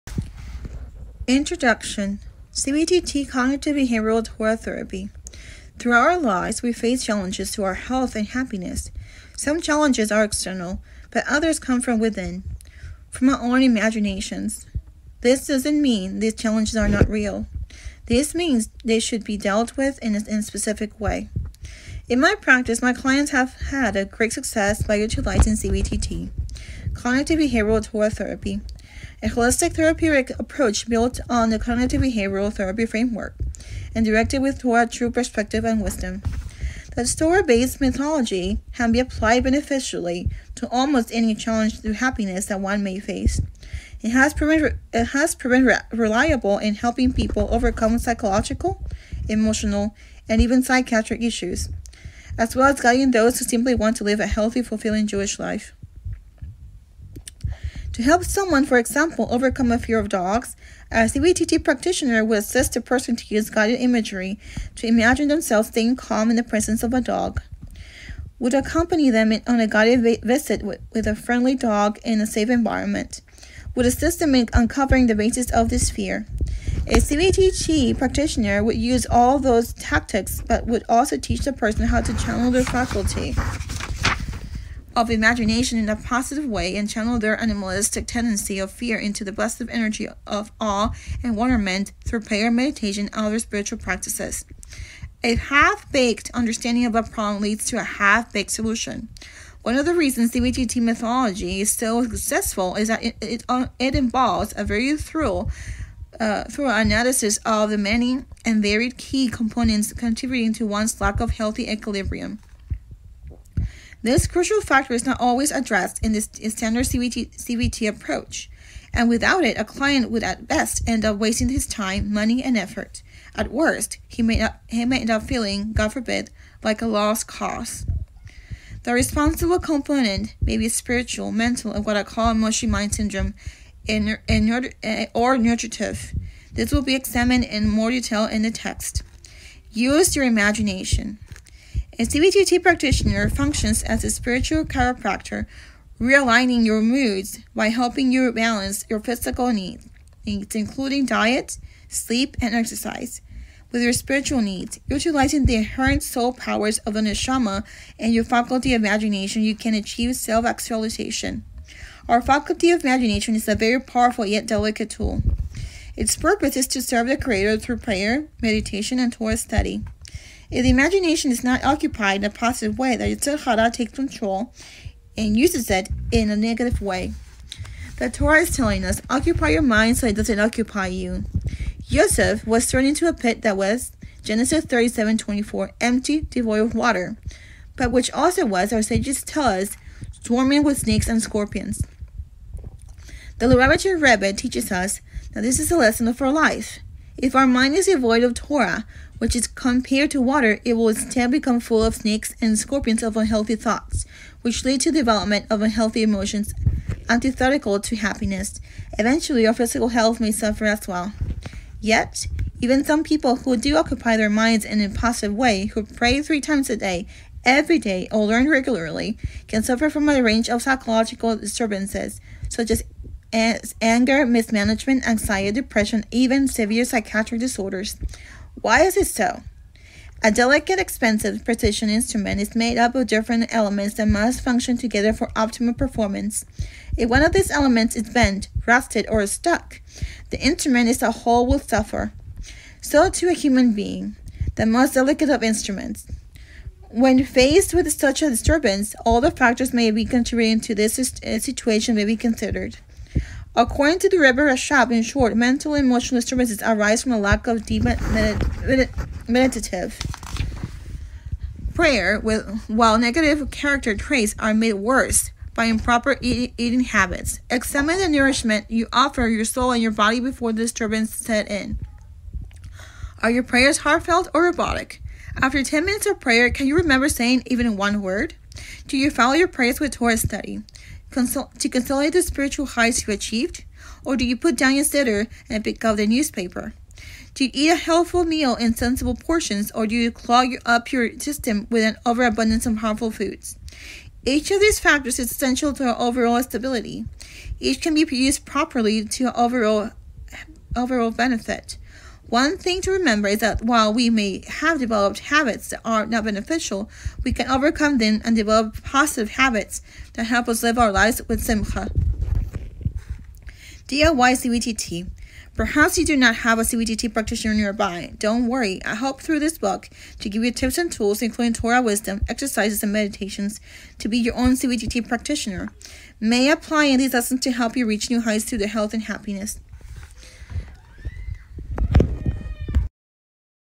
Reaching New Heights Through Health and Happiness Book Reading. Introduction
Reaching New Heights Through Health and Happiness Book Reading. Introduction. Reaching New Heights Through Health and Happiness: utilizing CBTT(TM) Cognitive Behavioral Torah Therapy